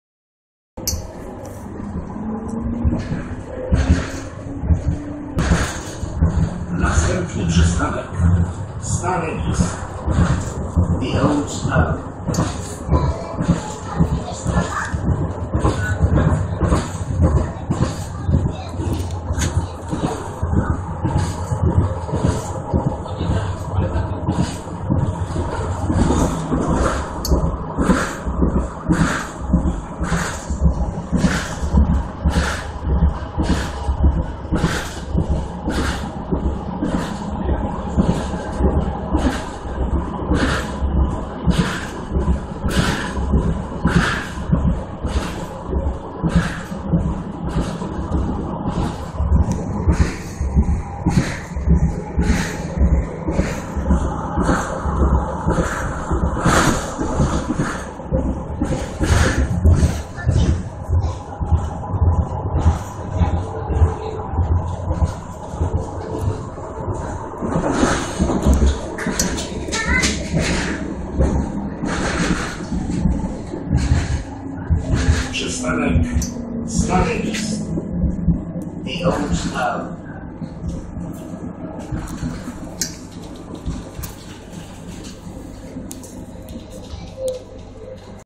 Warsaw, Poland up-close - view of the Vistula River, August 2016
Trolley ride across the Vistula River. Traveling west.